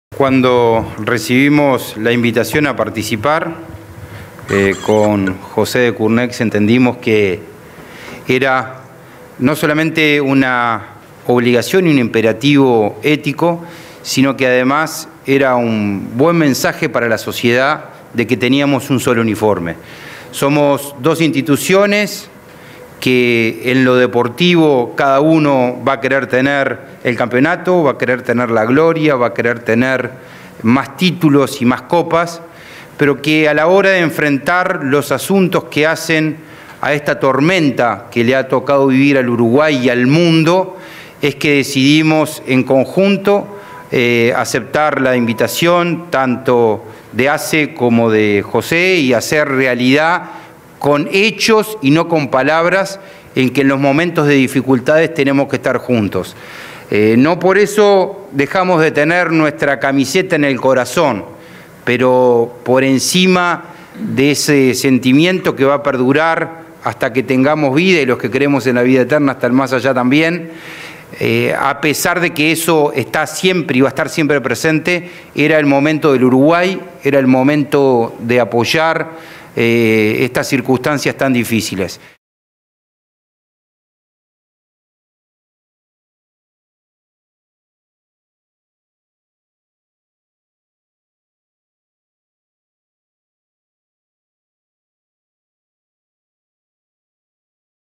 Jorge Barrera / Presidente de Peñarol
La campaña UnSoloUniforme, que integran el  Club Nacional de Fútbol y el Club Atlético Peñarol, entregó a ASSE 3000 mil equipos de protección personal para los trabajadores de la salud. Serán destinados al fortalecimiento del renovado Programa de Salud Bucal. En este momento de gran desafío para el personal de la salud, ambos equipos destacaron los valores solidarios más allá de camisetas.

El presidente de Peñarol, Jorge Barrera, expresó que más allá de la competencia deportiva entre ambos equipos, la colaboración es “una obligación y un imperativo ético”, a la hora “de enfrentar esta tormenta” que afecta a Uruguay y el mundo, “con hechos y no con palabras”.

Subrayó que es el momento de “estar juntos”, y que cuando pase la pandemia, “quede lo mejor de nosotros mismos”, como es “la solidaridad y el ser agradecidos”; al tiempo que destacó el esfuerzo de muchos uruguayos y del personal de salud, “que está en la primera línea de batalla”.

Por su parte, el presidente de Nacional, José Decurnex, afirmó que “más allá de camisetas y colores”, lo importante es dar a la sociedad uruguaya “un mensaje de unión y solidaridad”, así como de “los valores y las cosas que importan”, apoyando al país “y a todos los uruguayos”.

Afirmó que se trata de “un granito de arena” y una primera etapa, ya que la idea es “seguir apoyando y colaborando”.

Por su parte, el Presidente de ASSE, Dr. Leonardo Cipriani, resaltó que “no es solo un granito de arena”, ya que estos equipos representan “muchísimo para la protección de nuestros funcionarios”.

Hizo especial hincapié, en el simbolismo del hecho, dado que dos clubes que se enfrentan en una cancha como clásicos rivales, “se unen para dar el combate” al coronavirus; combate que afirmó “no está ganado”, más allá de que los números de infectados vienen bajando en relación al número de estudios realizados. “El virus está circulando y hay que estar siempre atentos”, aseveró.

El jerarca señaló que estos equipos de protección personal, serán destinados al renovado Programa de Salud Bucal de ASSE, que cuenta con una nueva Dirección, a cargo del Dr. Néstor Graña, que está reorganizando la atención de ese importante servicio en todo el país, con el objetivo de brindar la mayor cobertura a los usuarios del organismo.

Cipriani expresó que “crisis son oportunidades”, y valoró la gran colaboración de todo el pueblo uruguayo.

Participaron de la entrega de la donación, el Vicepresidente de ASSE, Marcelo Sosa, el Gerente General, Eduardo Henderson y otras autoridades del organismo.
Corresponsal en el Departamento de Colonia de TNU - VTV
Corresponsal en Juan Lacaze PUNTO OESTE
🎙MÚSICA EN EL AIRE
📻 89.1 FM 👉 Lunes a Viernes de 08:00 a 10:00